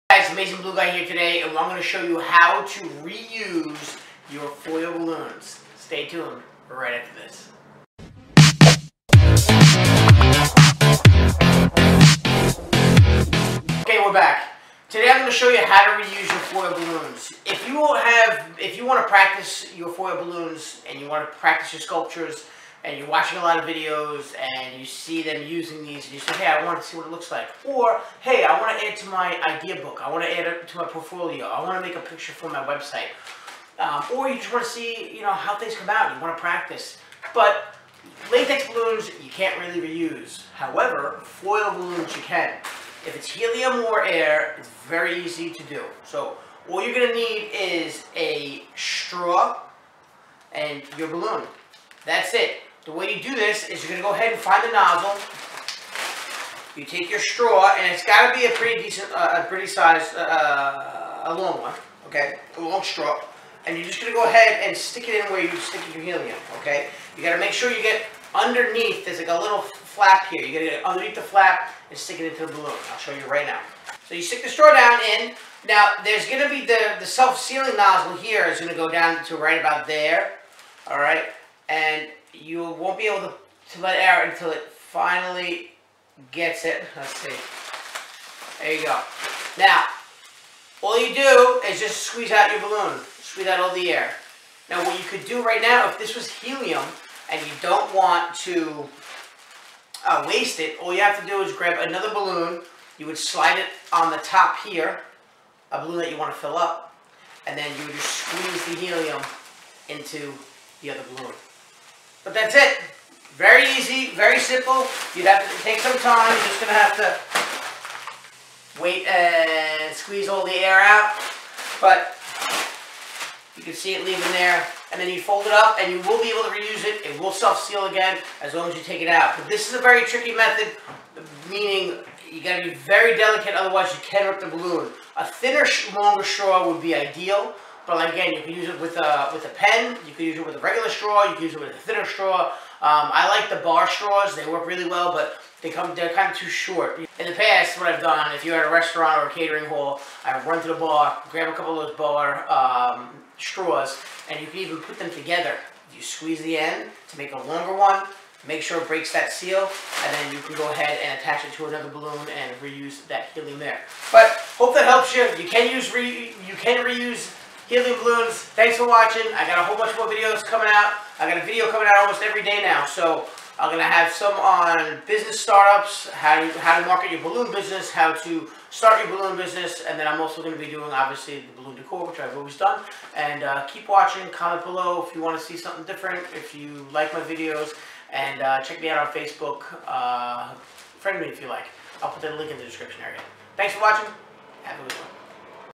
how to reuse your helium amd foil and mylar balloons
Save helium and reuse your foil and mylar balloons. Simple method of taking out air and helium from balloons. You can also transfer the gas from one balloon to the other